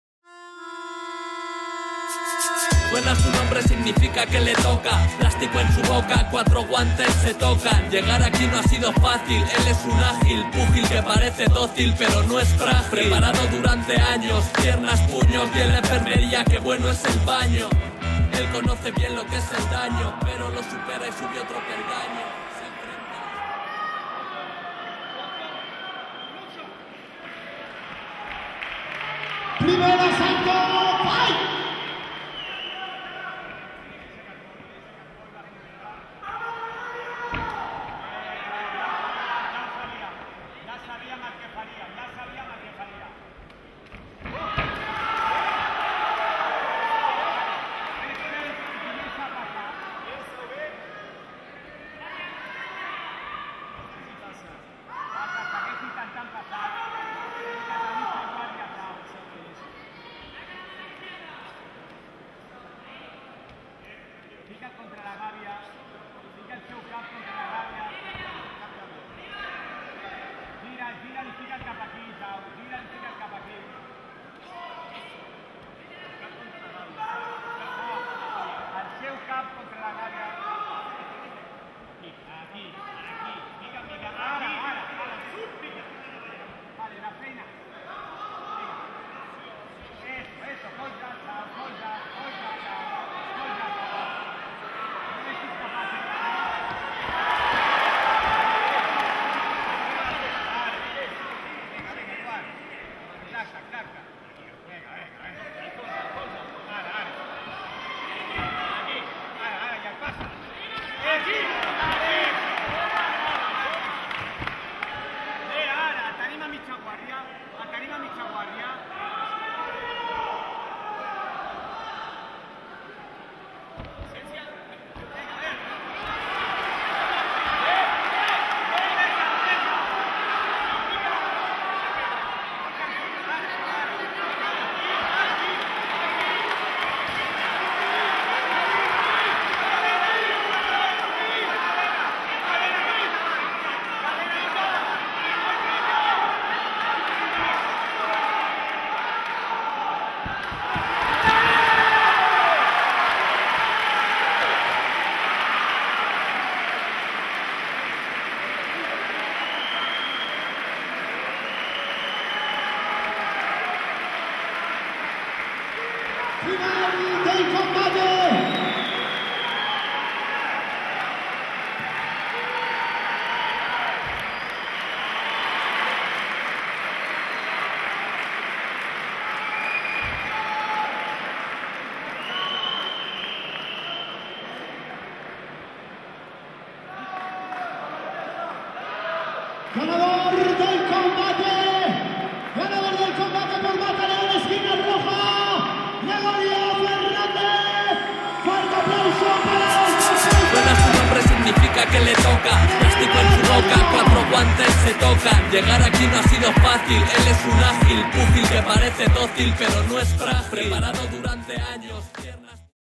AFL 5 BARCELONA | Zaurbek Dryaev vs Gregorio Fernandez
Combate de MMA de AFL-5 BARCELONA entre  Zaurbek Dryaev ( MMA Barcelona) y Gregorio Fernández (Camm ZGZ) el pasado 3 de Octubre 2015.

◾Web: aflmma.es